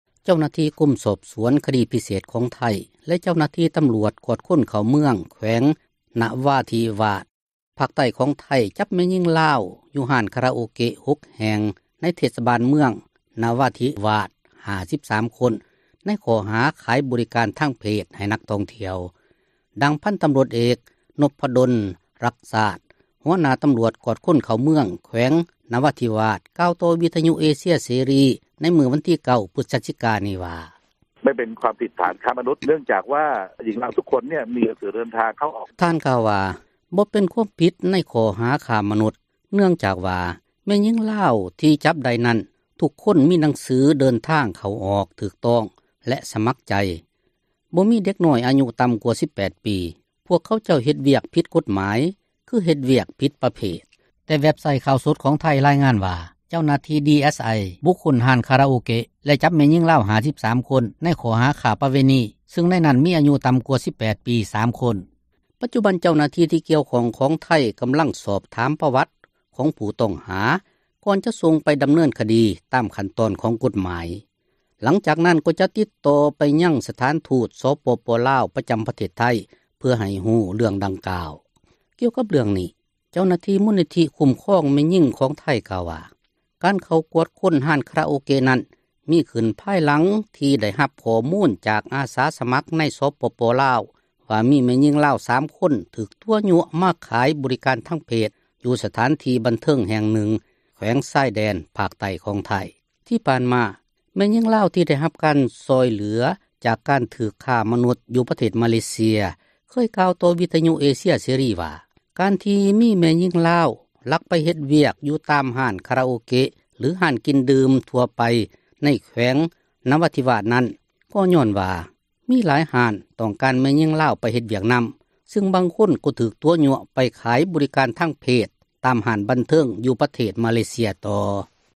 ຍິງສາວລາວ53 ຄົນ ຖືກຈັບ ຢູ່ໄທ
ເຈົ້າໜ້າທີ່ກົມສອບສວນຄະດີ ພິເສດຂອງໄທ ແລະເຈົ້າໜ້າທີ່ຕໍາຣວດ ກວດຄົນເຂົ້າເມືອງ ແຂວງນະຣາທິວາດ ພາກໃຕ້ຂອງໄທ ຈັບ ແມ່ຍິງລາວ ຢູ່ຮ້ານຄາຣາໂອເກະ 6 ແຫ່ງ ໃນເທດສະບານ ເມືອງນະຣາທິວາດ 53 ຄົນ ໃນຂໍ້ຫາຂາຍບໍຣິການ ທາງເພດ ໃຫ້ ນັກທ່ອງທ່ຽວ, ດັ່ງພັນຕໍາຣວດເອກ ນົບພະດົນ ຣັກຊາດ ຫົວໜ້າຕໍາຣວດກວດຄົນເຂົ້າເມືອງ ແຂວງນະຣາທິວາດ ກ່າວຕໍ່ວິທຍຸເອເຊັຍເສຣີ ໃນມື້ວັນທີ 9 ພຶສຈິກາ ນີ້ວ່າ:
"ທ່ານກ່າວວ່າ ບໍ່ເປັນຄວາມຜິດໃນຂໍ້ຫາຄ້າມະນຸດ ເນື່ອງຈາກວ່າ ແມ່ຍິງລາວ ທີ່ຈັບໄດ້ນັ້ນ ທຸກຄົນມີໜັງສືເດີນທາງ ເຂົ້າ-ອອກ ຖືກຕ້ອງ ແລະສມັກໃຈ, ບໍ່ມີເດັກນ້ອຍອາຍຸ ຕໍ່າກວ່າ 18 ປີ. ພວກເຂົາເຈົ້າເຮັດວຽກຜິດກົດໝາຍ ຄືເຮັດວຽກຜິດປະເພດ."
ແຕ່ທາງ ເວັບໄຊ ຂ່າວສົດຂອງໄທ ຣາຍງານວ່າເຈົ້າໜ້າທີ່ ດີເອສໄອ ບຸກຄົ້ນຮ້ານຄາຣາໂອເກະ ແລະຈັບແມ່ຍິງລາວ 53 ຄົນ ໃນຂໍ້ຫາ ຄ້າປະເວນີ ຊຶ່ງໃນນັ້ນມີອາຍຸຕໍ່າກວ່າ 18 ປີ 3 ຄົນ.
ປັດຈຸບັນ ເຈົ້າໜ້າທີ່ກ່ຽວຂ້ອງຂອງໄທ ກໍາລັງສອບປວັດຜູ້ຕ້ອງຫາຢູ່ ກ່ອນຈະສົ່ງໄປດໍາເນີນຄະດີ ຕາມຂັ້ນຕອນຂອງກົດໝາຍ ຫຼັງຈາກ ນັ້ນກໍຈະຕິດຕໍ່ໄປຍັງ ສະຖານທູດ ສປປລາວ ປະຈໍາປະເທສໄທ ເພື່ອໃຫ້ຮັບຮູ້ໃນເຣຶ່ອງດັ່ງກ່າວ